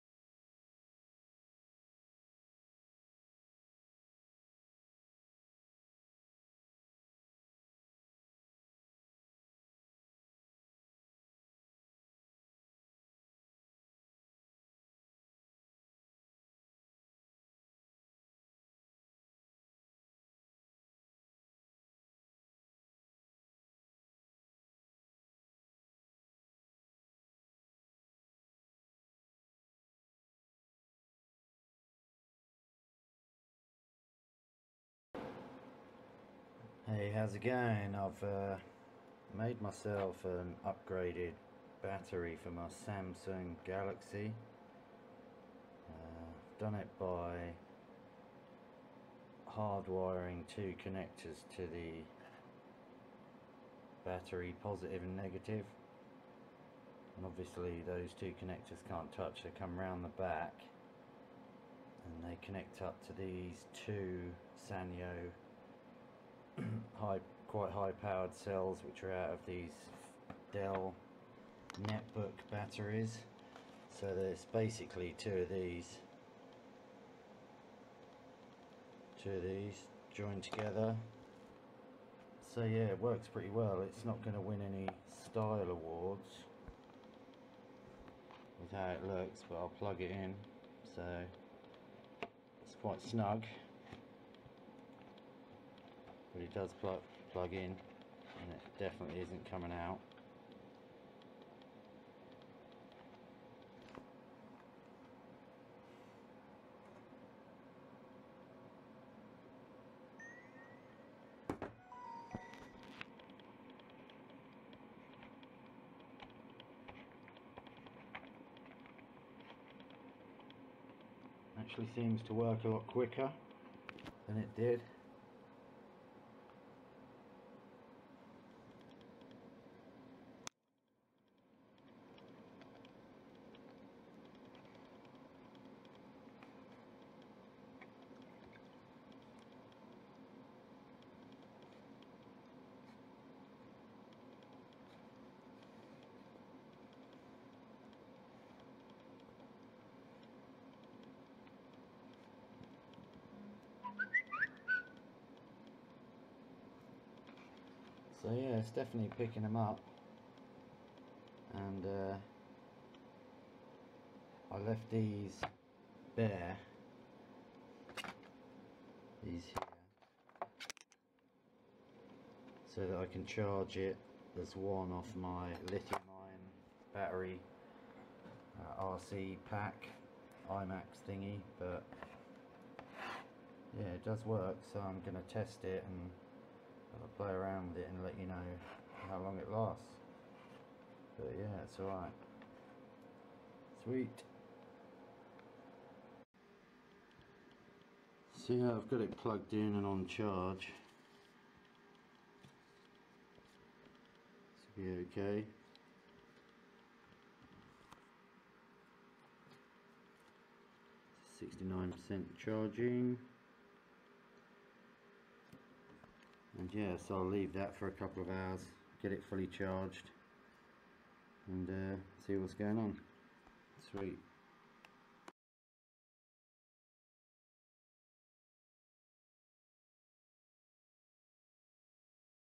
Samsung Galaxy S3 DIY Easy triple battery upgrade hack lithium ion recycling $5 cost
This is a triple battery hack for the Samsung Galaxy S3,I want to prolong the life of my phone battery also I want to be able to charge it via the USB and via an imax b6 charger.

This is very easy to make..

The battery cells for the upgrade cost me less than 1£ each they are Sanyo from the Dell 8hour netbook batteries, they are very light weight and have good power, the phone actually seems to run faster.